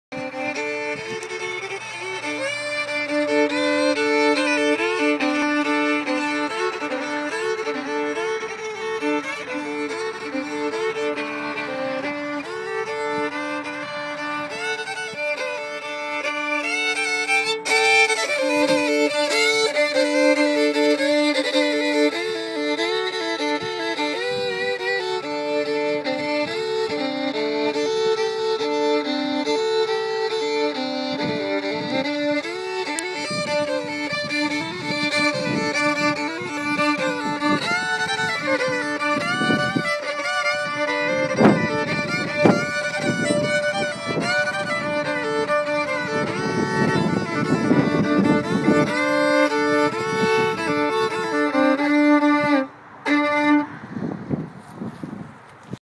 Smitty the Giant Fiddler dwarfed by giant guitar
Defibulators fiddler Justin Smith aka. The Giant Fiddler drags the bow beneath the Grand Guitar museum/radio station in Bristol TN.